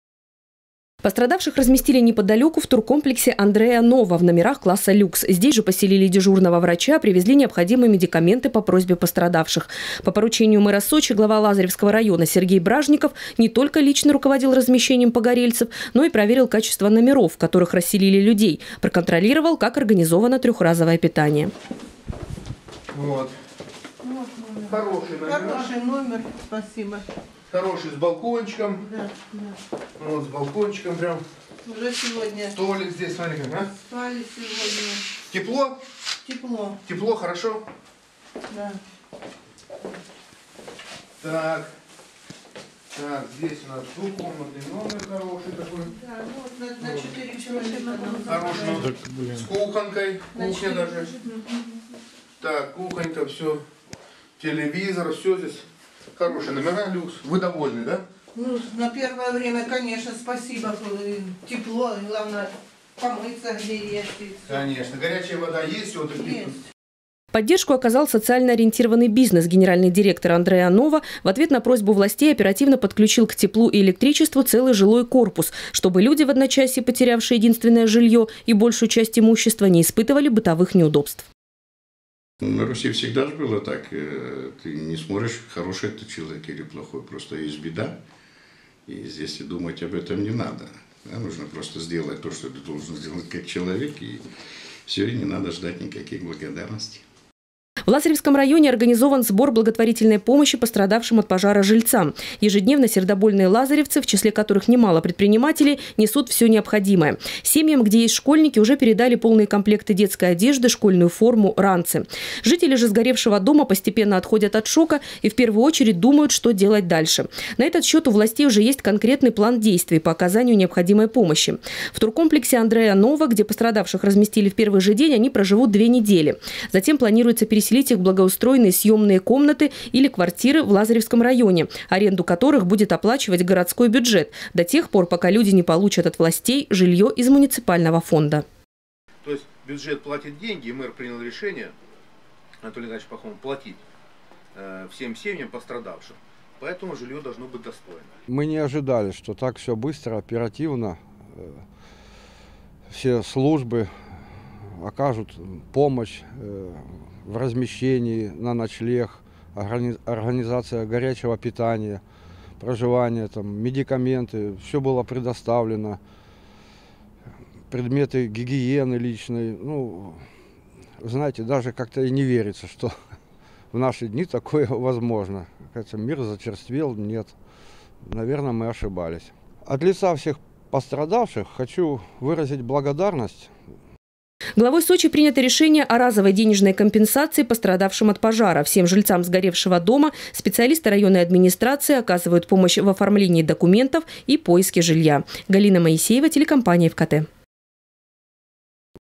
Погорельцы из Макопсе получат денежные компенсации. Новости Эфкате Сочи
Администрации Сочи и Лазаревского района продолжают мероприятия по оказанию помощи жильцам сгоревшего в Макопсе десятиквартирного жилого дома.

Пострадавших разместили неподалеку в туркомплексе «АндреаНова» в номерах класса «люкс». Здесь же поселили дежурного врача, привезли необходимые медикаменты по просьбе пострадавших. По поручению мэра Сочи, глава Лазаревского района Сергей Бражников не только лично руководил размещением погорельцев, но и проверил качество номеров, в которых расселили людей, проконтролировал, как организовано 3-разовое питание.

Поддержку оказал социально ориентированный бизнес. Генеральный директор «АндреаНова» Дмитрий Аручиди в ответ на просьбу властей оперативно подключил к теплу и электричеству целый жилой корпус, чтобы люди, в одночасье потерявшие единственное жильё и большую часть имущества, не испытывали бытовых неудобств.

В Лазаревском районе организован сбор благотворительной помощи пострадавшим от пожара жильцам. Ежедневно сердобольные лазаревцы, в числе которых немало предпринимателей, несут самое необходимое. Семьям, где есть школьники, уже передали полные комплекты детской одежды, школьную форму, ранцы. Жители же сгоревшего дома постепенно отходят от шока и в первую очередь думают, что делать дальше. На этот счёт у властей уже есть конкретный план действий по оказанию необходимой помощи. В туркомплексе «АндриаНова», где пострадавших разместили в первый же день, они проживут две недели. Затем планируется переселить их в благоустроенные съемные комнаты или квартиры в Лазаревском районе, аренду которых будет оплачивать городской бюджет, до тех пор, пока люди не получат от властей жильё из муниципального фонда.

Сочи. Новости.
Телеканал Эфкате.